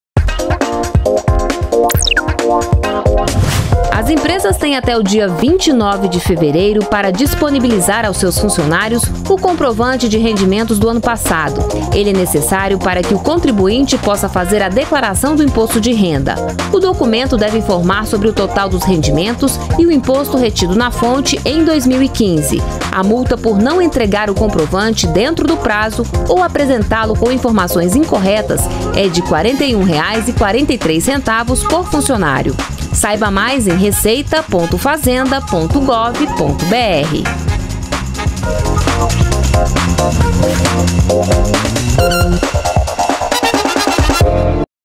Empresas devem entregar comprovante de rendimentos de 2015 até o dia 29 de fevereiro
FIQUE ATENTO - 25.02.16: Empresas devem entregar para seus funcionários o comprovante de rendimentos de 2015 até o dia 29 de fevereiro. O comprovante é necessário para que o contribuinte faça a declaração do imposto de renda deste ano. Empresas que não entregarem o comprovante dentro do prazo ou apresentá-lo com informações incorretas receberão multa de R$ 41,43 por funcionário. Para mais informações, acesse o site: receita.fazenda.gov.br.